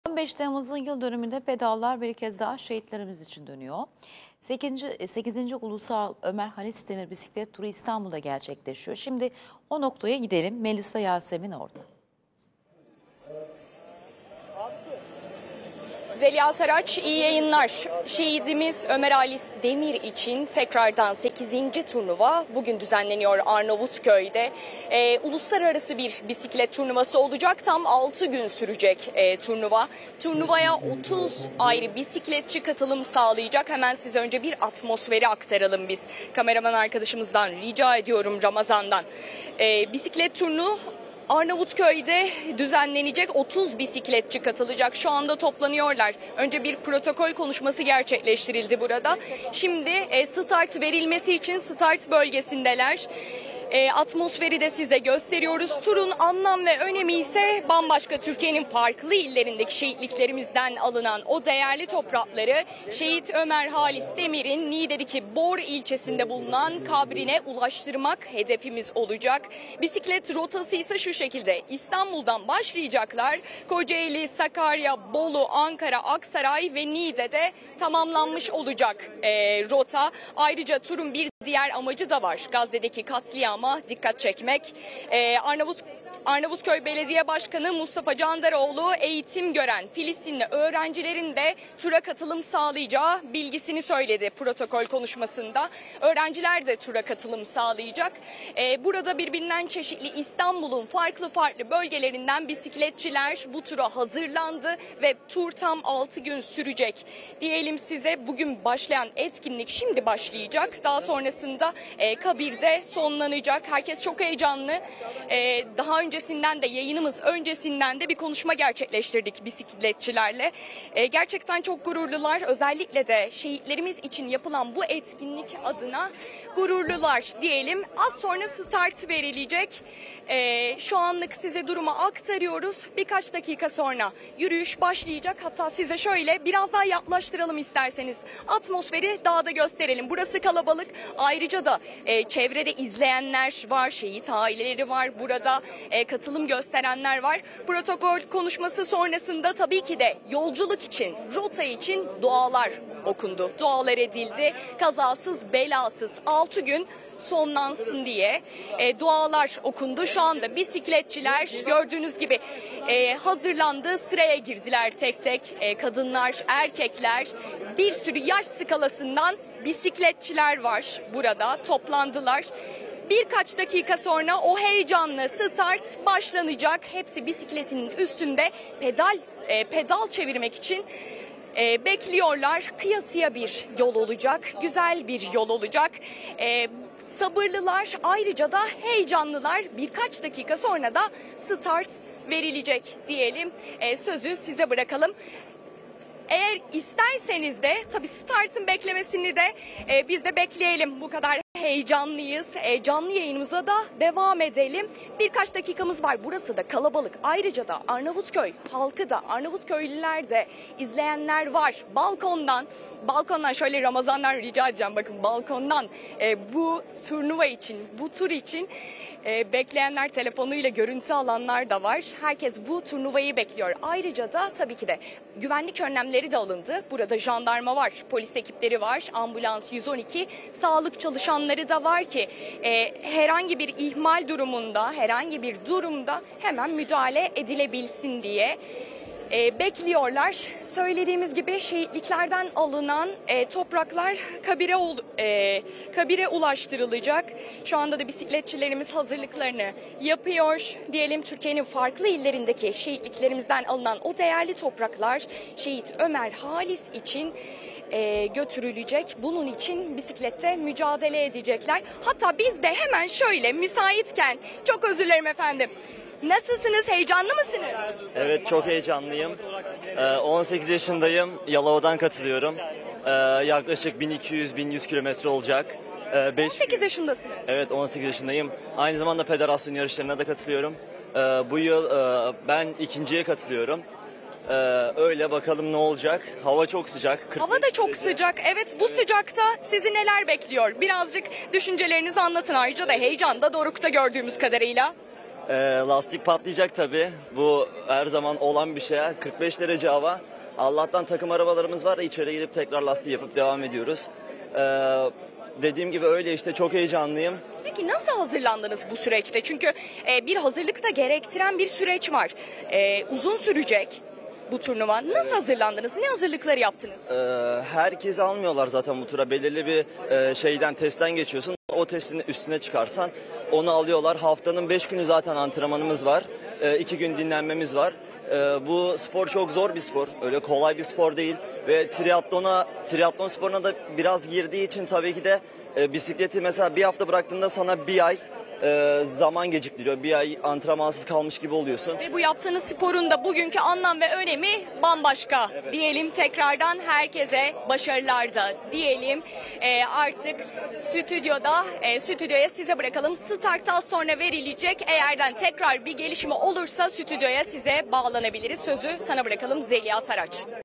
Ömer Halisdemir 8. Ulusal Bisiklet Turu!
Türkiye'nin dört bir yanından gelen 30 bisikletçi, Türkiye genelindeki birçok ilin şehitliklerinden alınan toprakları şehit Ömer Halisdemir'in Niğde'nin Bor ilçesinde bulunan kabrine ulaştıracak. Bisikletçiler tur boyunca İstanbul, Kocaeli, Sakarya, Bolu, Ankara, Aksaray ve Niğde illerinden geçecek. Turun startı, 10 Temmuz saat 10.00'da Arnavutköy 15 Temmuz Demokrasi Meydanı'ndan verilecek.

Digiturk 49
D-smart 39
Kablo Tv 72
Tivibu 60
Tv+ 47